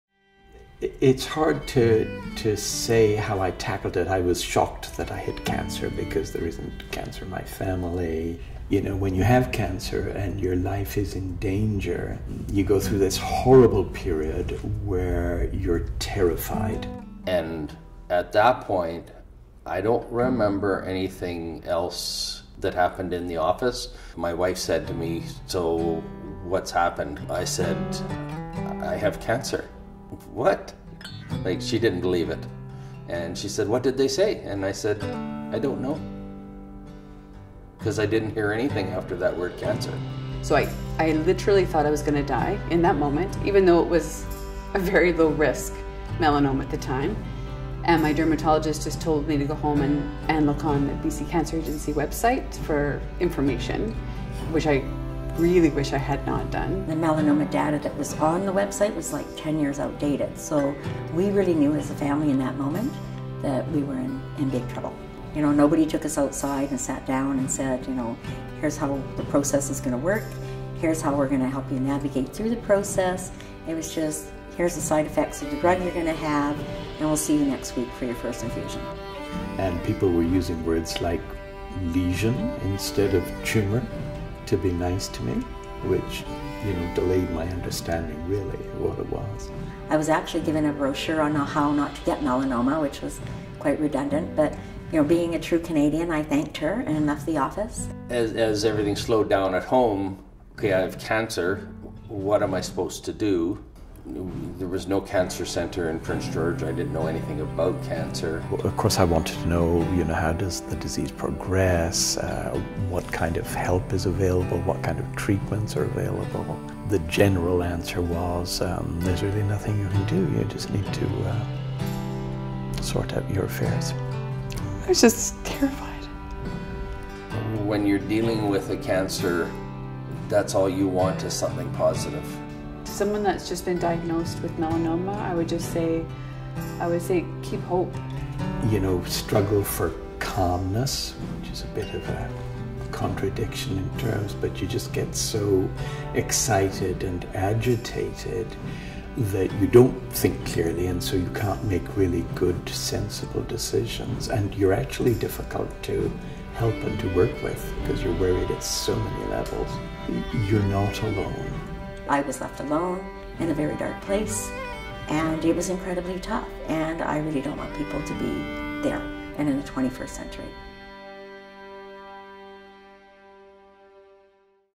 Melanoma Patient Perspective: The Emotional Impact of a Cancer Diagnosis
While dealing with a cancer diagnosis, many patients report that dealing with their emotional wellness is as important as their physical wellness. And yet, this area of treatment is lacking for many.

Hear four melanoma survivors discuss the emotional impacts of their cancer diagnosis.